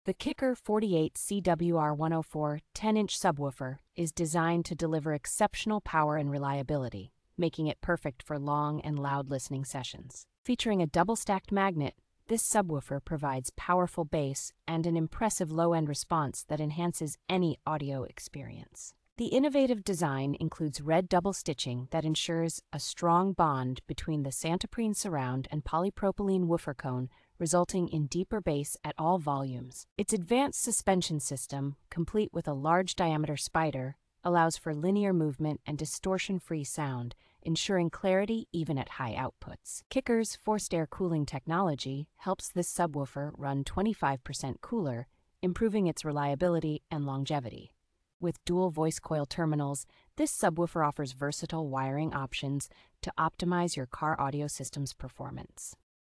REVIEW (2024): KICKER 48CWR104 10" Subwoofer. ESSENTIAL details.
The KICKER 48CWR104 ten inch subwoofer is designed to deliver exceptional power and reliability, making it perfect for long and loud listening sessions.

Featuring a double-stacked magnet, this subwoofer provides powerful bass and an impressive low-end response that enhances any audio experience.

The innovative design includes red double stitching that ensures a strong bond between the Santoprene surround and polypropylene woofer cone, resulting in deeper bass at all volumes.

Its advanced suspension system, complete with a large-diameter spider, allows for linear movement and distortion-free sound, ensuring clarity even at high outputs.

KICKER's Forced-Air Cooling technology helps this subwoofer run twenty-five percent cooler, improving its reliability and longevity.

With dual voice coil terminals, this subwoofer offers versatile wiring options to optimize your car audio system's performance.